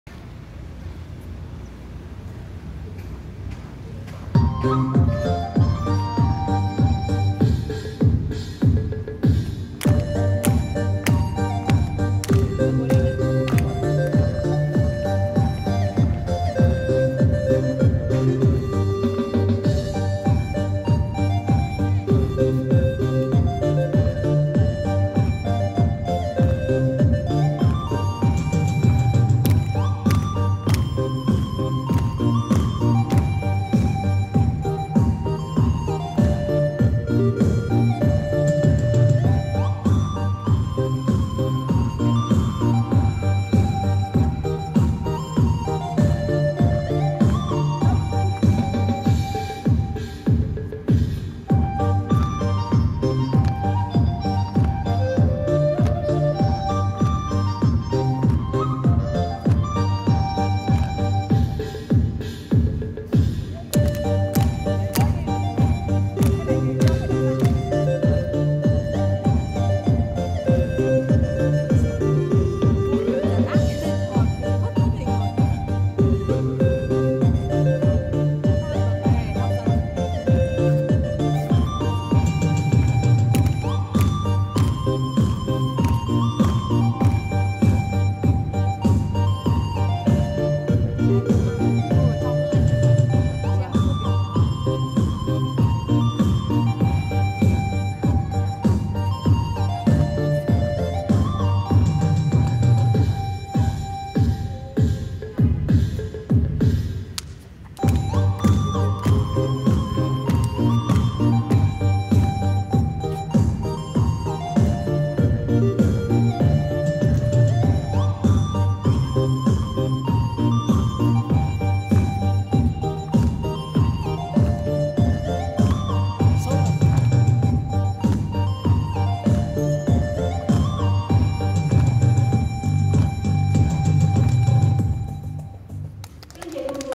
Giáo viên trường người ta nhảy đẹp - Vũ điệu vùng cao - Trường tiểu học Hữu Bằng
Trường tiểu học Hữu Bằng, Kiến Thụy, Hải Phòng